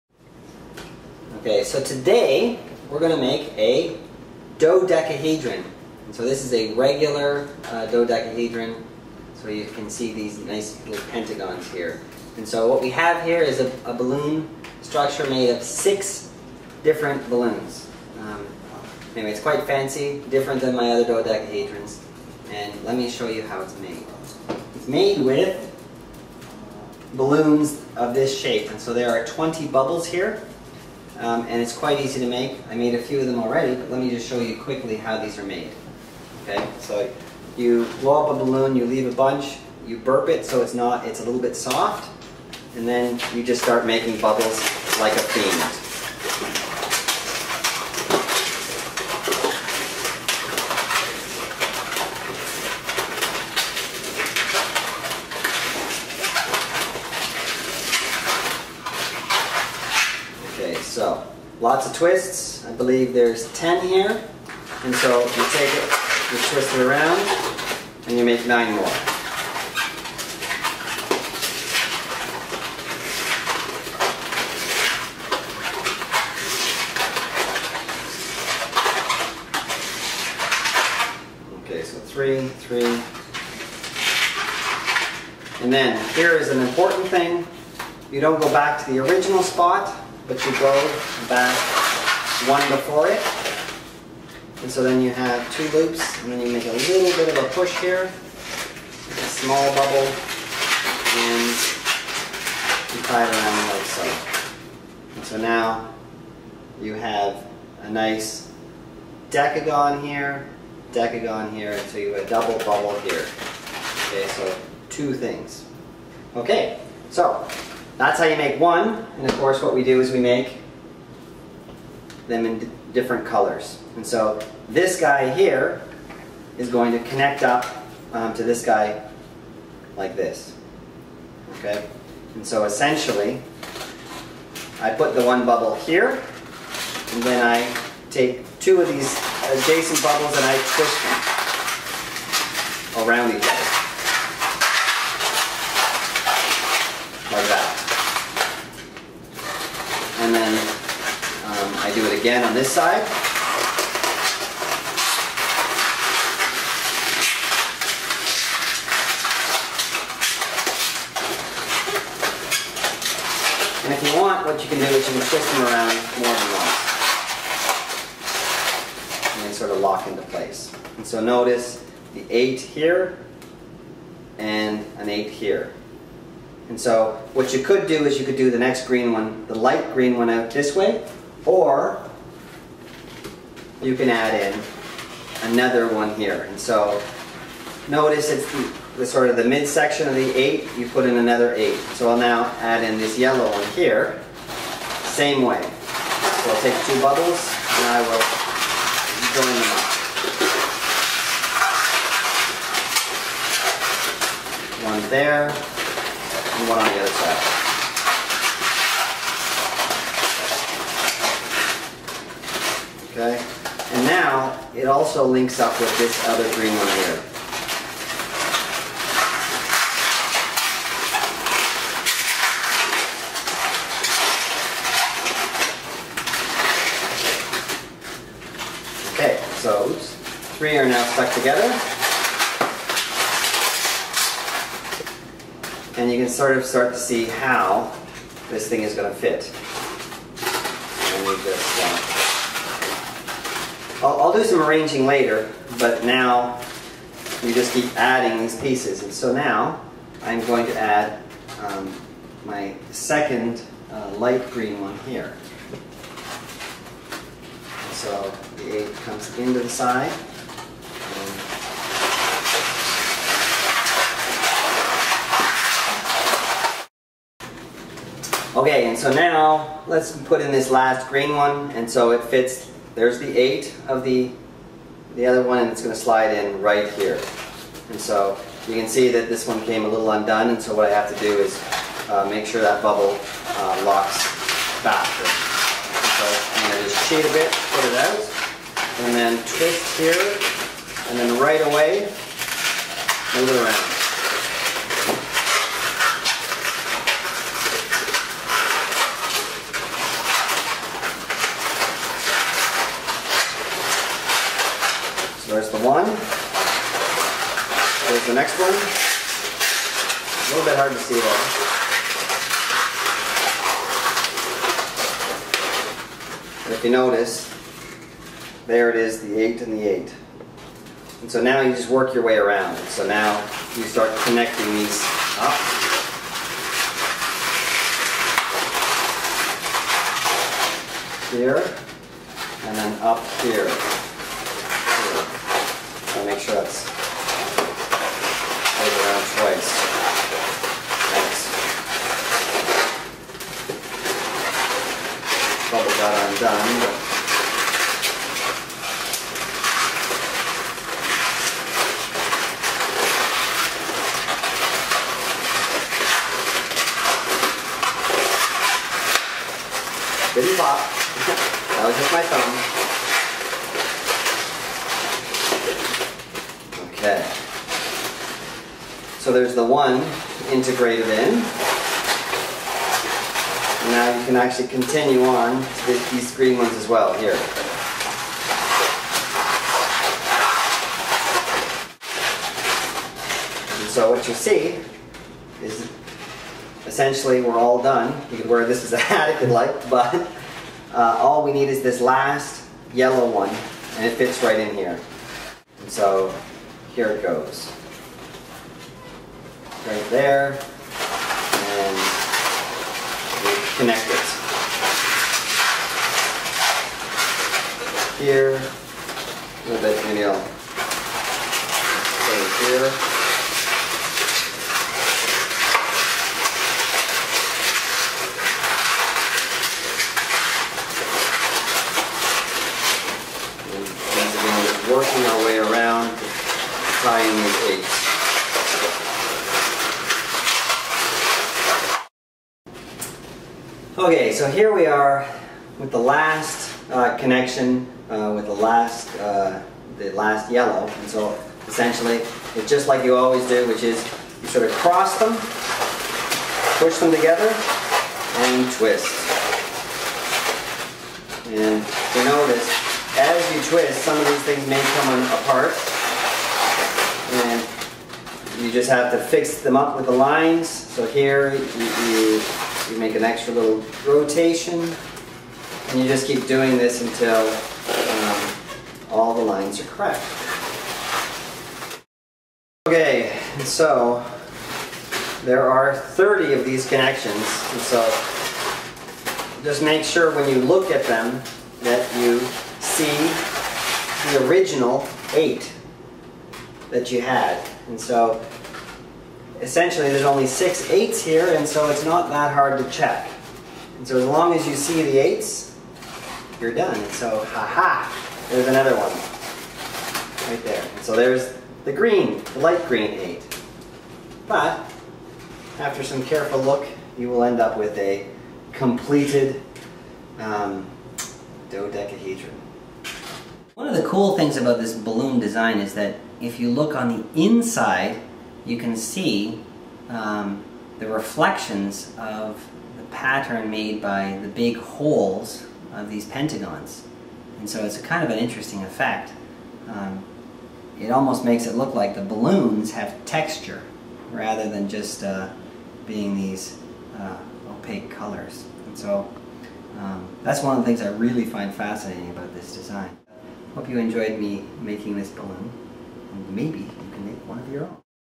Fancy 6-Balloon Dodecahedron
A 120 bubble dodecahedron is made with 6 balloons.  It's modular design helps reduce the hassle of having one of the balloons pop.  Technically, the design is a truncated dodecahedron with some doubled up edges but it looks more like a dodecahedron.